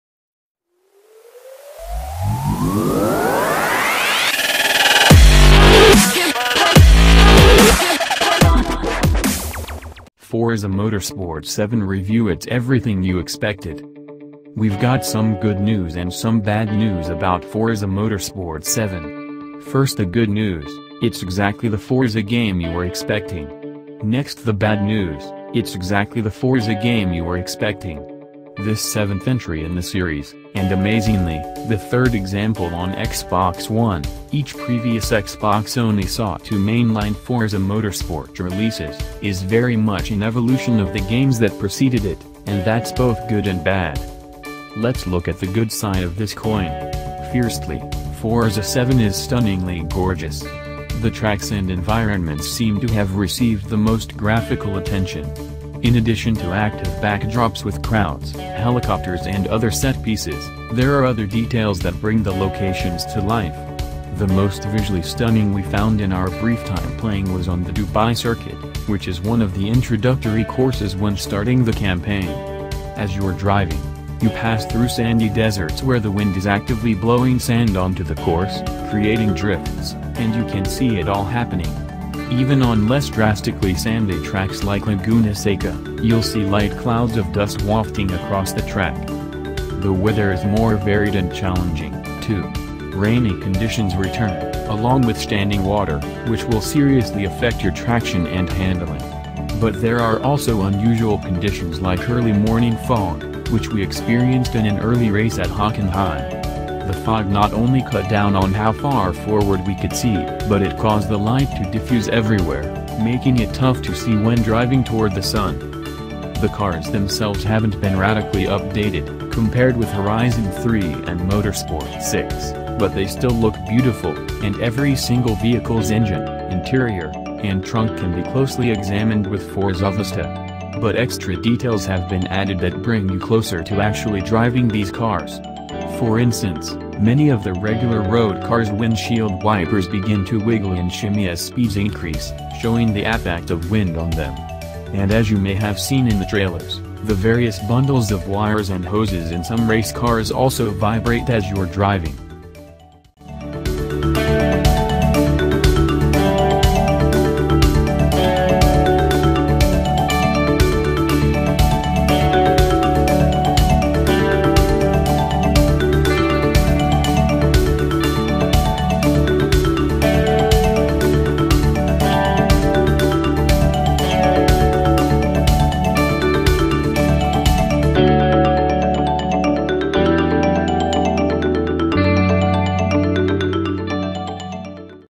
Forza Motorsport 7 review It's everything you expected
We've got some good news and some bad news about Forza Motorsport 7. First the good news: It's exactly the Forza game you were expecting. Next the bad news: It's exactly the Forza game you were expecting. This seventh entry in the series, and amazingly, the third example on Xbox One (each previous Xbox only saw two mainline Forza Motorsport releases), is very much an evolution of the games that preceded it, and that's both good and bad.

Let's look at the good side of this coin. Firstly, Forza 7 is stunningly gorgeous. The tracks and environments seem to have received the most graphical attention. In addition to active backdrops with crowds, helicopters and other set pieces, there are other details that bring the locations to life. The most visually stunning we found in our brief time playing was on the Dubai circuit, which is one of the introductory courses when starting the campaign. As you're driving, you pass through sandy deserts where the wind is actively blowing sand onto the course, creating drifts, and you can see it all happening. Even on less drastically sandy tracks like Laguna Seca, you'll see light clouds of dust wafting across the track.

The weather is more varied and challenging, too. Rainy conditions return, along with standing water, which will seriously affect your traction and handling. But there are also unusual conditions like early morning fog, which we experienced in an early race at Hockenheim. The fog not only cut down on how far forward we could see, but it caused the light to diffuse everywhere, making it tough to see when driving toward the sun.

The cars themselves haven't been radically updated, compared with Horizon 3 and Motorsport 6, but they still look beautiful, and every single vehicle's engine, interior, and trunk can be closely examined with Forzavista. But extra details have been added that bring you closer to actually driving these cars. For instance, many of the regular road cars' windshield wipers begin to wiggle and shimmy as speeds increase, showing the affect of wind on them. And as you may have seen in the trailers, the various bundles of wires and hoses in some race cars also vibrate as you're driving.